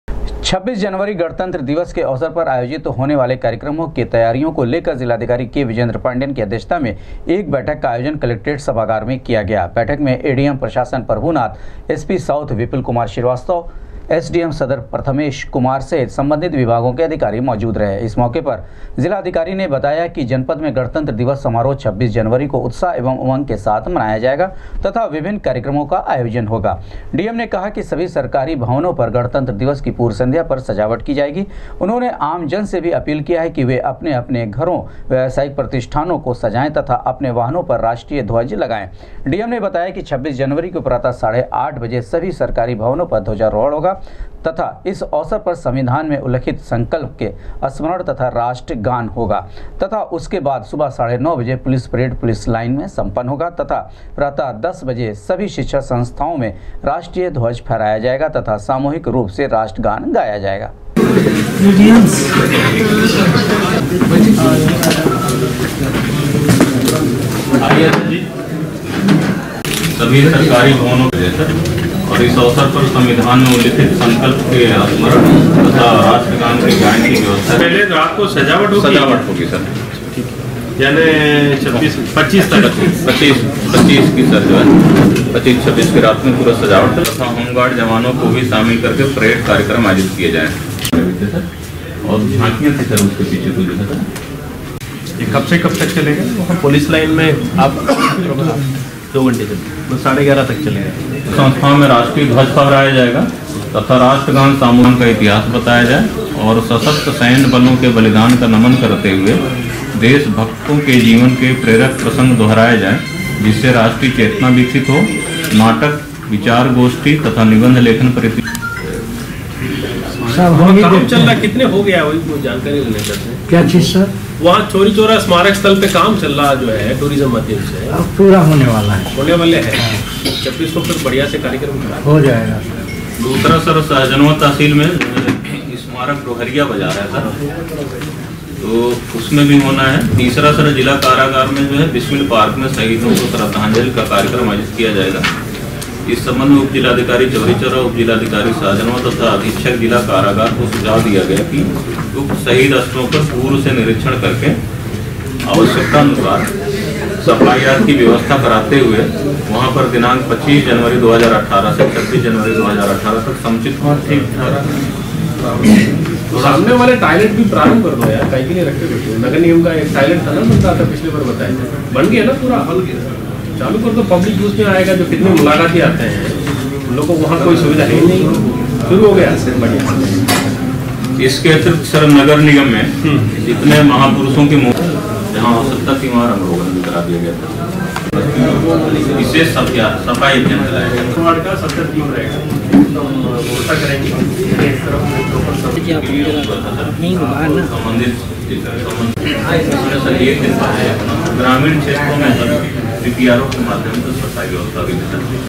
जिलाधिकारी की अध्यक्षता में सम्पन्न हुई गणतंत्र दिवस समारोह की तैयारी बैठक, सरकारी
जिलाधिकारी की अध्यक्षता में सम्पन्न हुई गणतंत्र दिवस समारोह की तैयारी बैठक, सरकारी तथा गैर सरकारी कार्यालयों तथा अपने अपने घरों पर राष्ट्रीय ध्वज फहराने की कि अपील